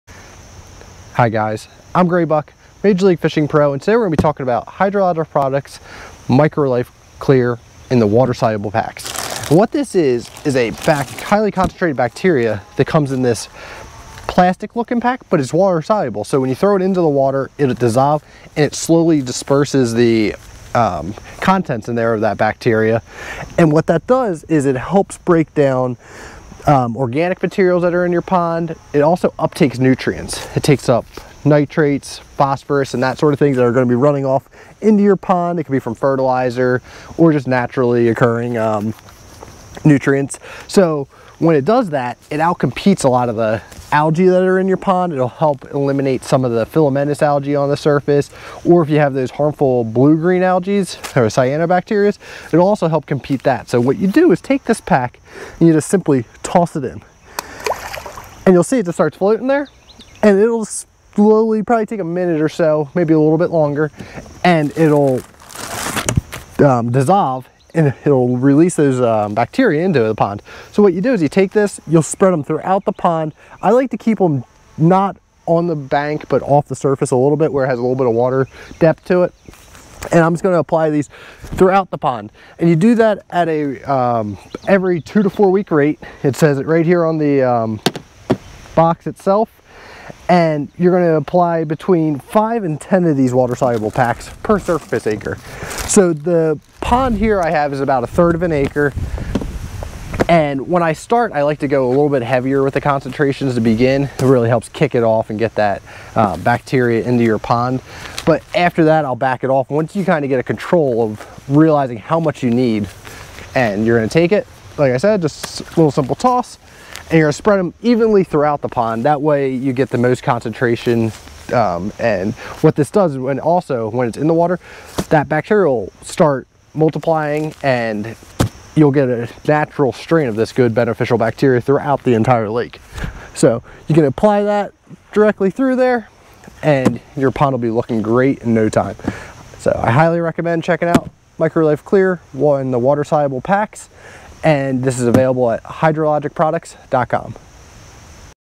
How to Clear Up Your Pond!- MicroLife Clear Pond Bacteria Water Soluble Packs
MicroLife Clear is an all natural formulation of concentrated beneficial bacteria for use in ponds, lakes, aquaculture ponds, ornamental ponds, koi ponds and water garden ponds. Apply this all season product around the edges of the pond when water temperature is above 55 degrees Fahrenheit (F). When used regularly, this product will reduce murky water (improve water clarity); reduce ammonia, nitrate, nitrite and phosphorus; reduce bottom sludge (muck); and reduce odors. Apply 6 to 8 packets per surface acre every 2 to 4 weeks. Available in large pail containing 50-8 oz. dissolvable packets.